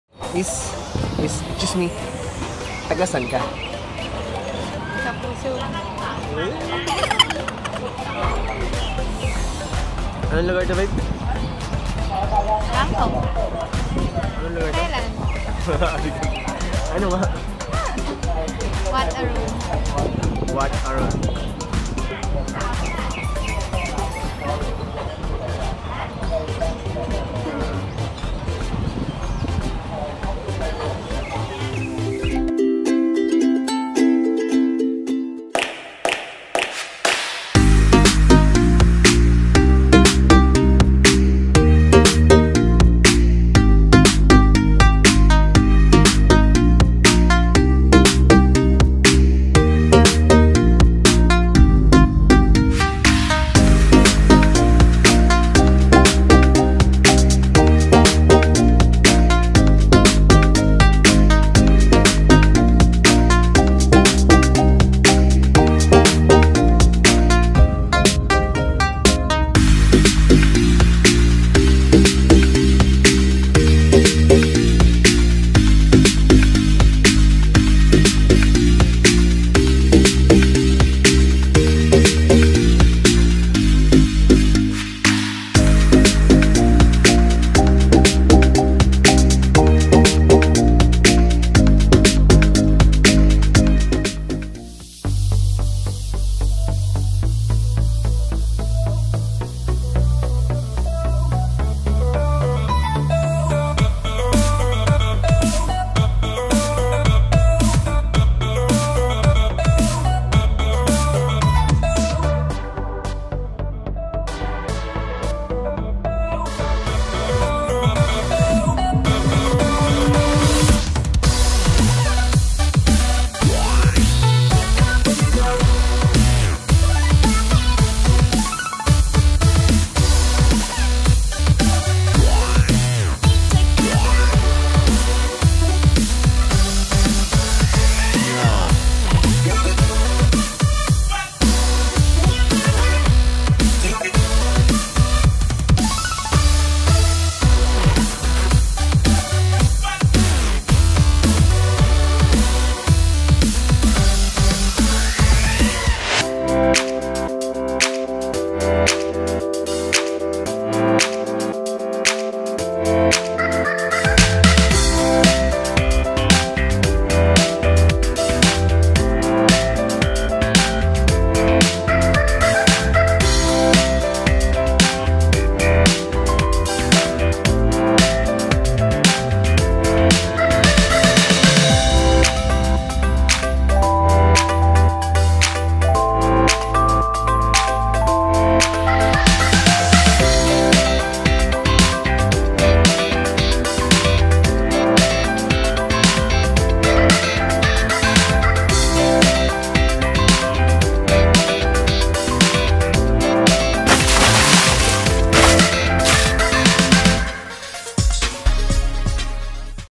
Bangkok
Bangkok, Thailand’s capital, is a large city known for ornate shrines and vibrant street life.

The boat-filled Chao Phraya River feeds its network of canals, flowing past the Rattanakosin royal district, home to opulent Grand Palace and its sacred Wat Phra Kaew Temple.

Nearby is Wat Pho Temple with an enormous reclining Buddha and, on the opposite shore, Wat Arun Temple with its steep steps and Khmer-style spire.

It is known in Thai as Krung Thep Maha Nakhon

✓It's more FUN if you share your thoughts, suggestions and opinions. Just drop it down in the comment section below